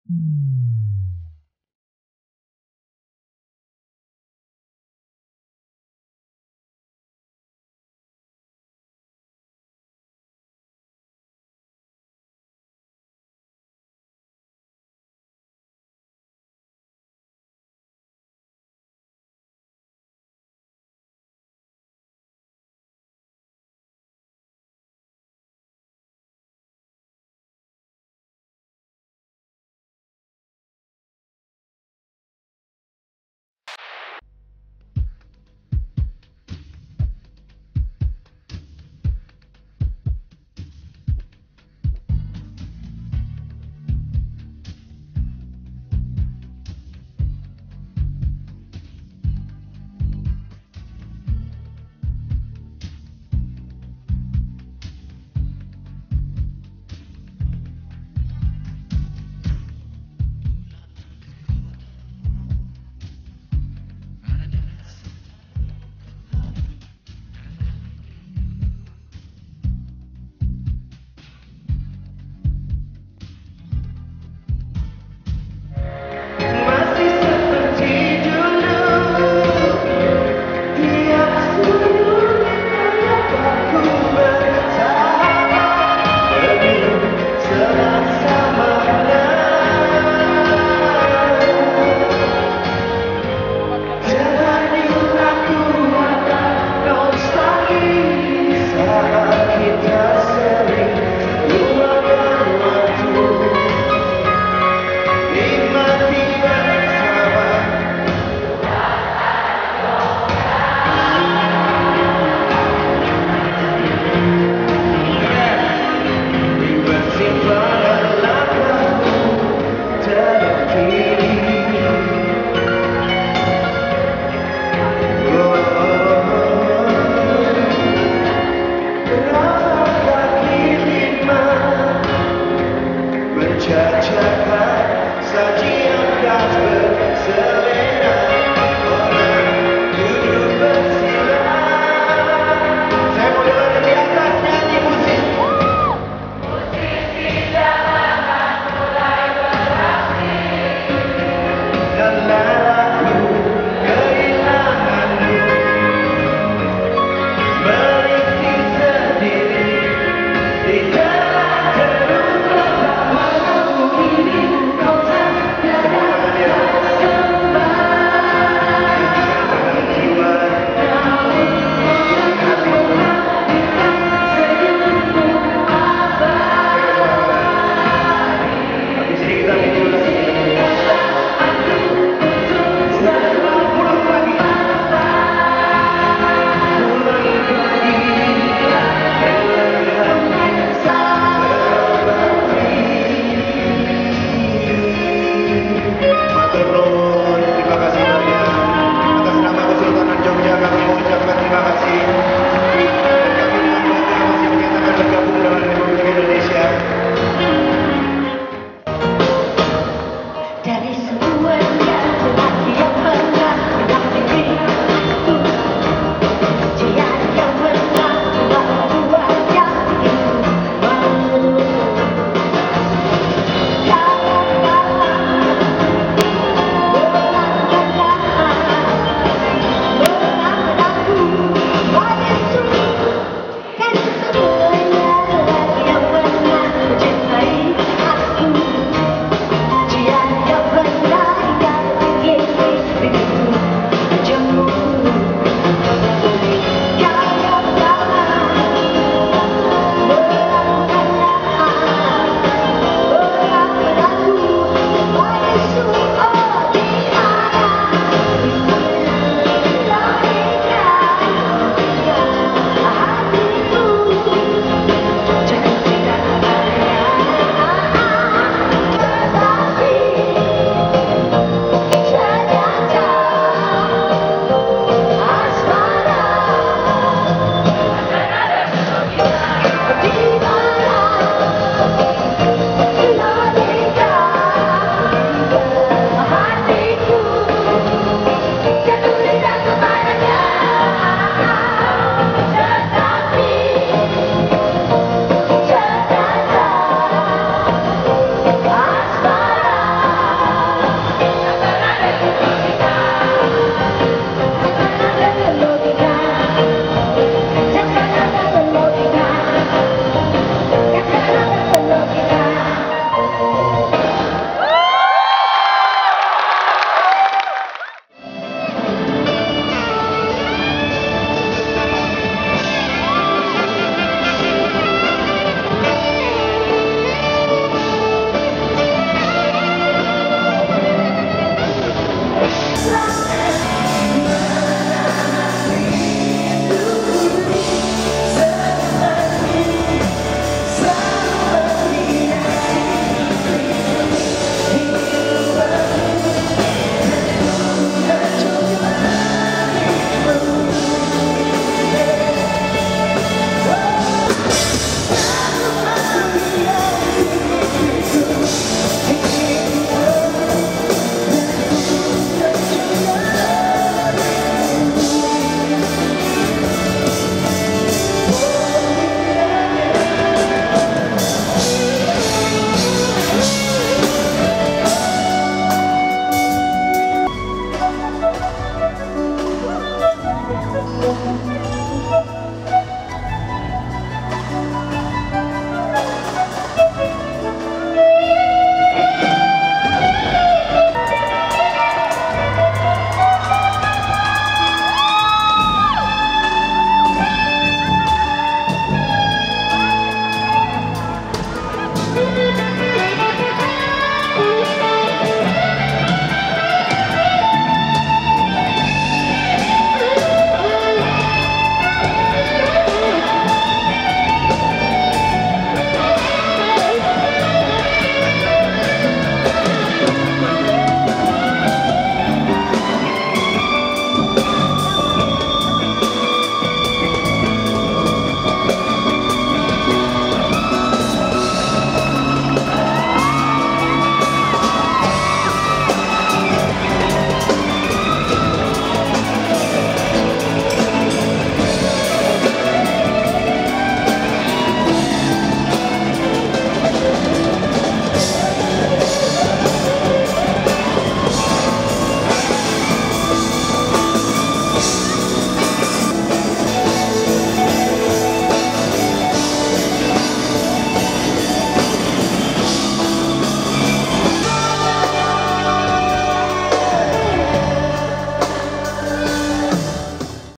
KONSER CINTA MUSIK INDONESIA
Vina Panduwinata, Kla Project dan Glenn Fredly menjadi pengisi gelaran bertajuk Konser Cinta Musik Indonesia pada Sabtu, 4 Mei 2013 di Tennis Indoor Senayan, Jakarta Pusat.